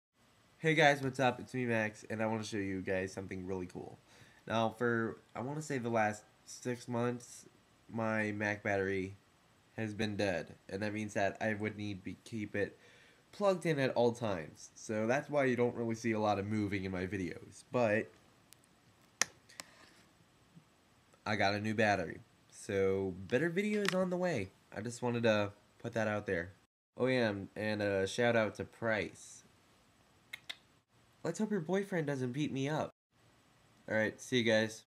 new battery
hey guys whatsup? so i have a new battery. ballin. just wanted to put that out there. also, im doing the burry vs binkx contest. im going for binkx, because although i cant spell his name right, i still like his videos. i actually fliped a coin to see which team i would be on, because both channels are AMAZING! ok c ya guyz.